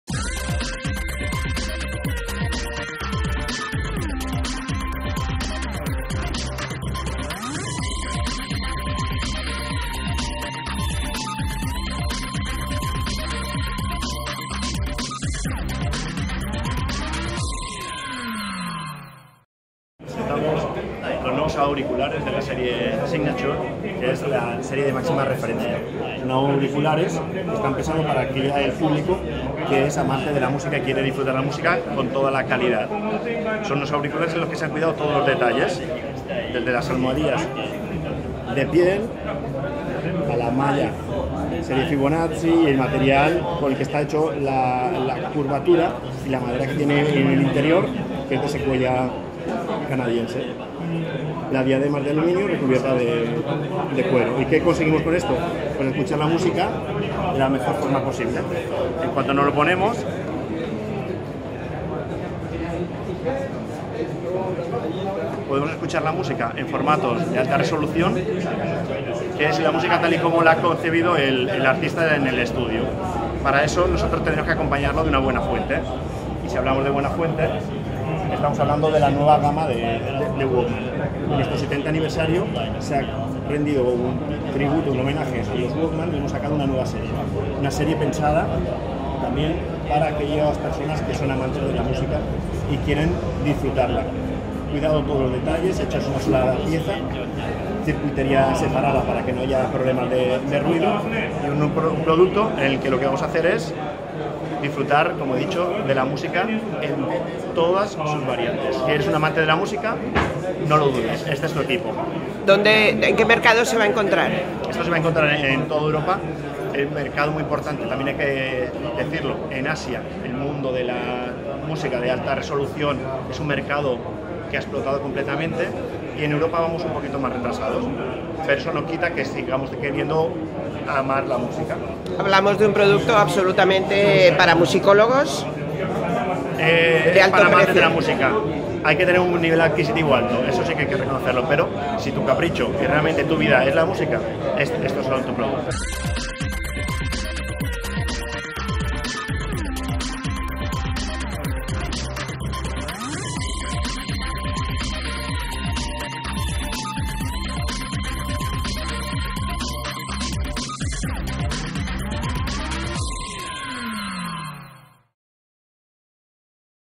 Nuevos Auriculares Sony Signature
Los auriculares MDR-Z1R tienen un amplio rango de frecuencias que ofrecen un escenario sonoro natural y abierto, con reproducción de hasta 120 kHz en audio de alta resolución